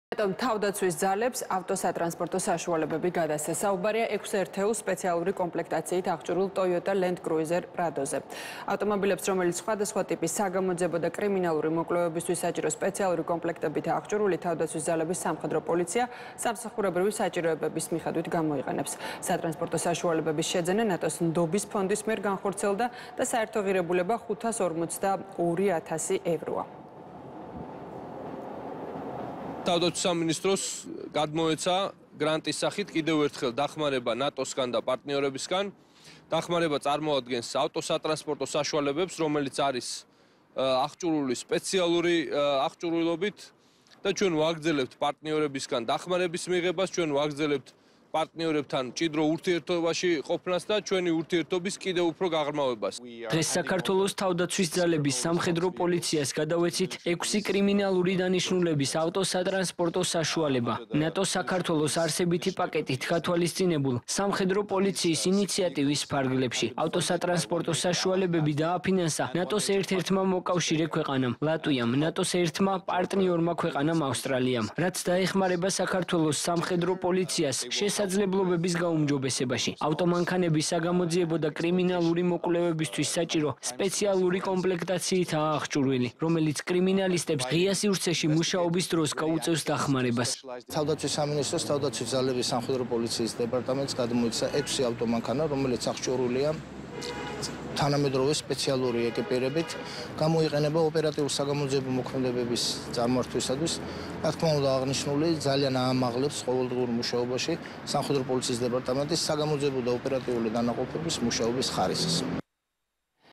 ნატო-მ თავდაცვის ძალებს ავტოსატრანსპორტო საშუალებები გადასცა
ნატო-მ თავდაცვის ძალებს ავტოსატრანსპორტო საშუალებები გადასცა. საუბარია ექვს ერთეულ სპეციალური კომპლექტაციით აღჭურვილ „ტოიოტა ლენდ კრუიზერ პრადოზე“. ავტომობილებს, რომელიც სხვადასხვა ტიპის საგამოძიებო და კრიმინალური მოკვლევებისთვის საჭირო სპეციალური კომპლექტებითაა აღჭურვილი, თავდაცვის ძალების სამხედრო პოლიცია სამსახურებრივი საჭიროებებისას გამოიყენებს.

სატრანსპორტო საშუალებების შეძენა ნატო-ს ნდობის ფონდის მიერ განხორციელდა და საერთო ღირებულება 542 ათას ევროს შეადგენს. პროექტის კონტრიბუტორები არიან ლატვია და ავსტრალია.